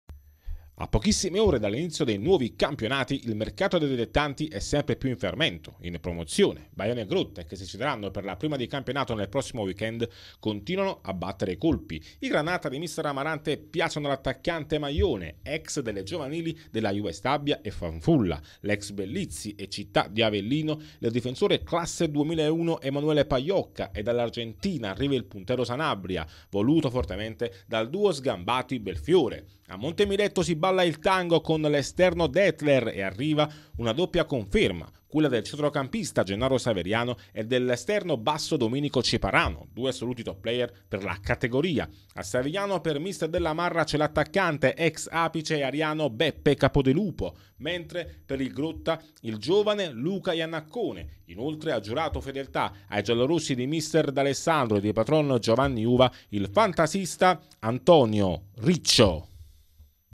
Baiano e Montemiletto ballano il tango con Sanabria e Dettler
Doppio colpo dall'Argentina per Lions e Baiano. Alla corte dei leoni ecco Dettler, mentre nel Mandamento arriva l'attaccante Sanabria